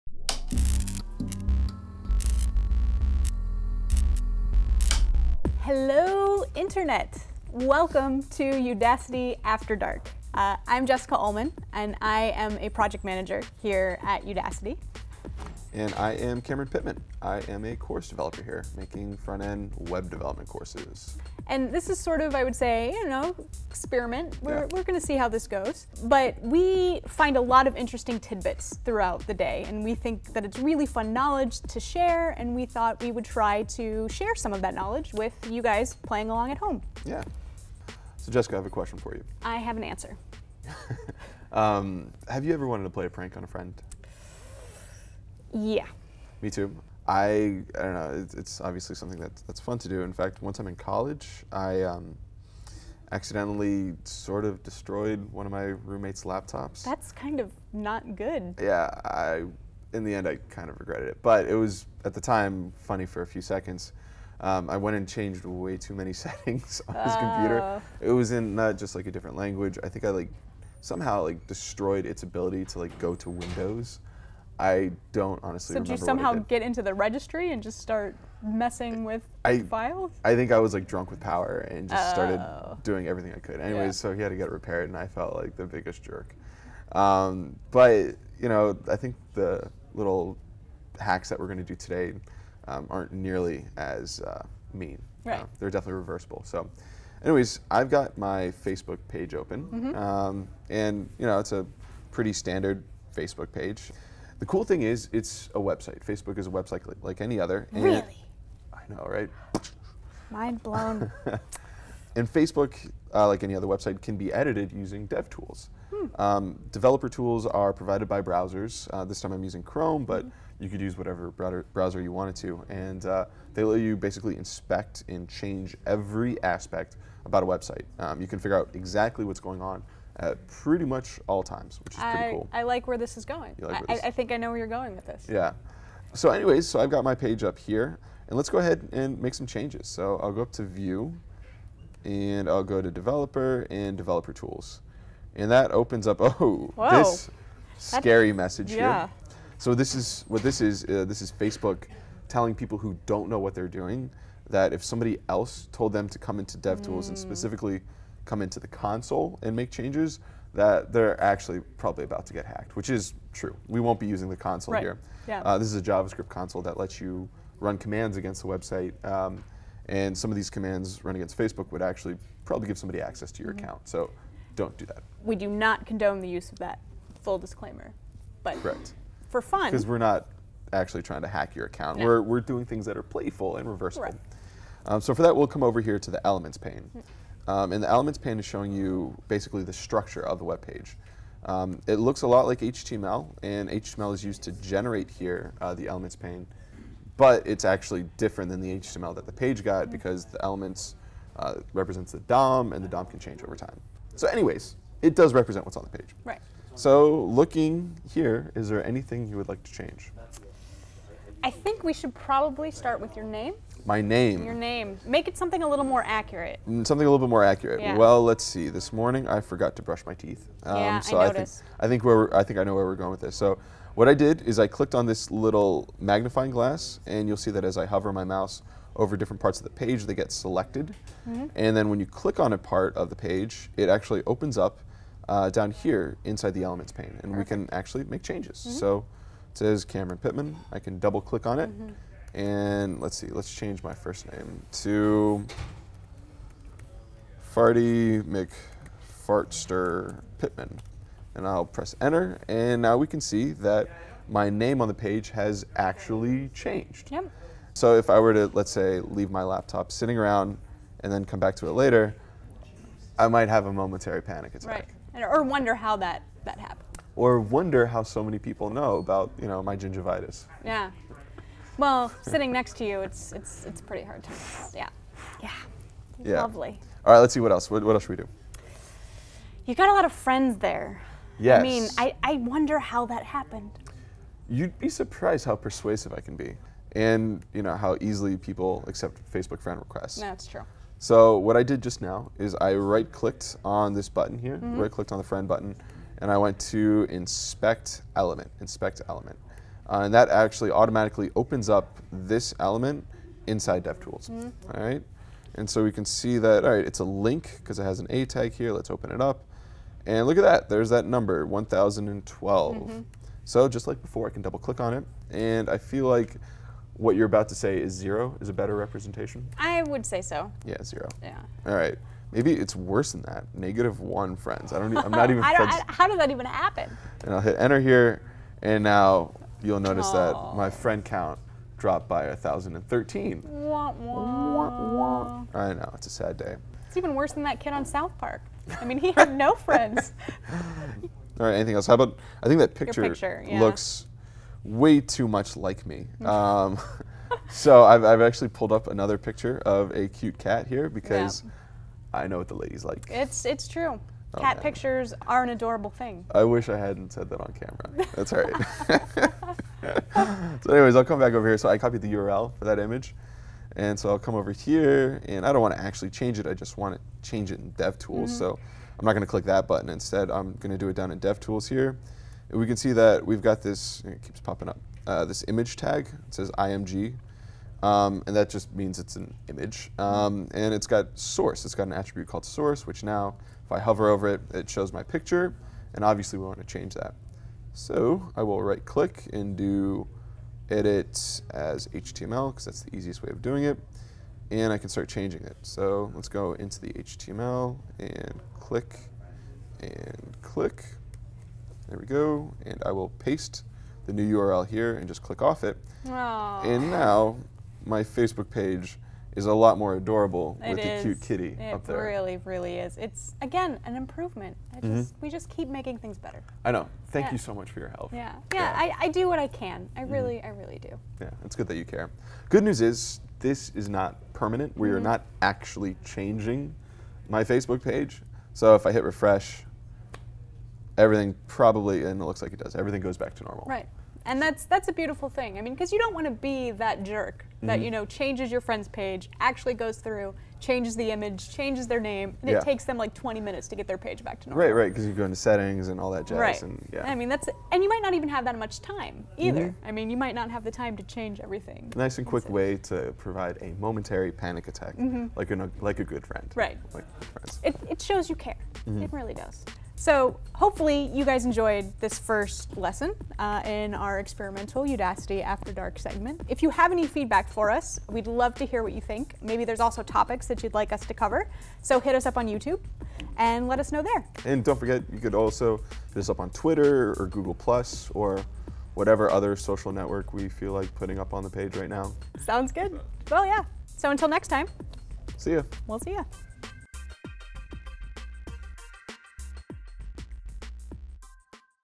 Udacity After Dark: "Hacking" Facebook
How to use Developer Tools to modify elements and "hack" a Facebook page.